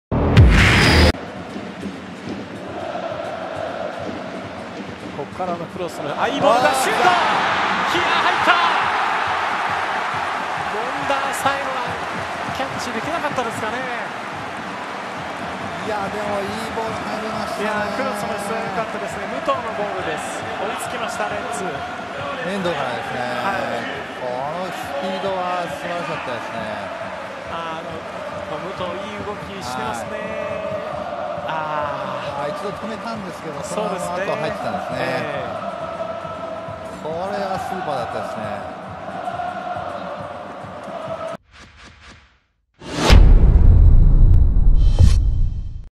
【公式】ゴール動画：武藤 雄樹（浦和）85分 浦和レッズvsサガン鳥栖 明治安田生命Ｊ１リーグ 第27節 2017/9/23
武藤 雄樹（浦和）が右サイドからのクロスボールを頭で合わせると、一度は阻まれるも自ら押し込んで同点弾！明治安田Ｊ１のゴール動画を公開中！
2017年9月23日（土）に行われた明治安田生命Ｊ１リーグ 第27節 浦和vs鳥栖のゴール動画です。